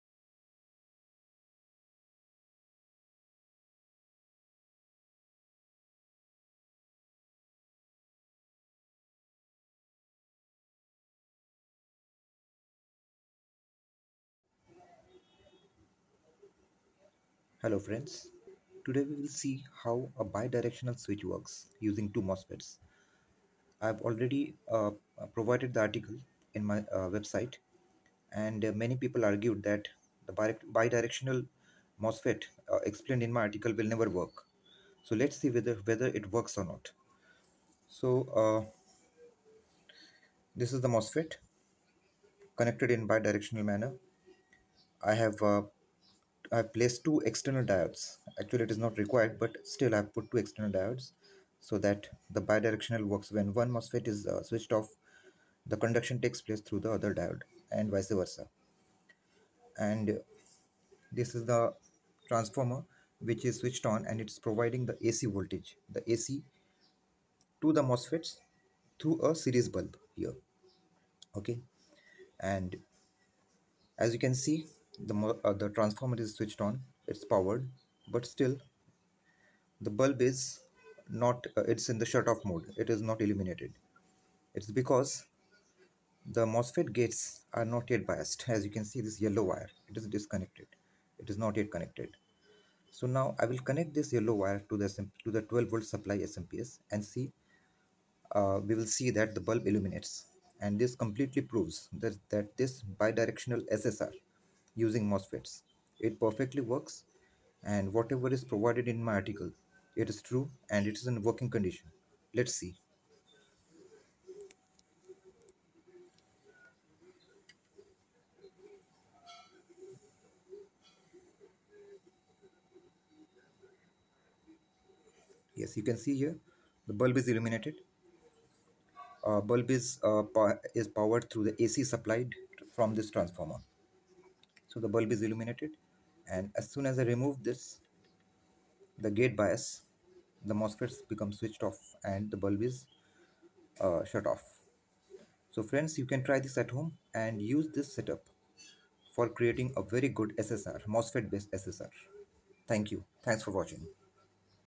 Testing a Solid State SSR using MOSFET Bidirectional Switch
The video shows the circuit diagram and the testing a simple solid state relay using MOSFET based Bidirectional Switch.